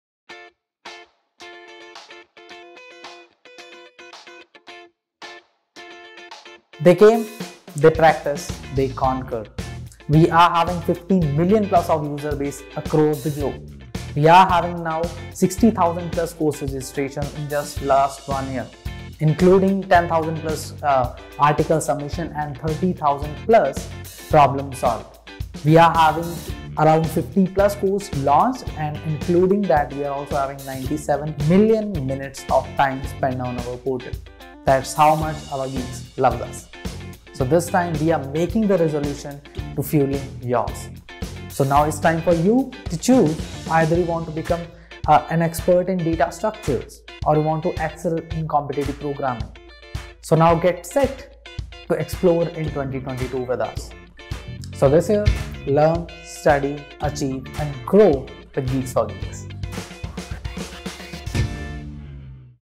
Resolution Days 2022 | GeeksforGeeks
A very happy new year 2022 to all our geeks! And with this new year, comes a whole lot of positivity and hope. And to celebrate this, GeeksforGeeks is proud to present to you- Resolution Days 2022.
Come and join us from 7th January onwards and get ready to learn, earn and have some fun!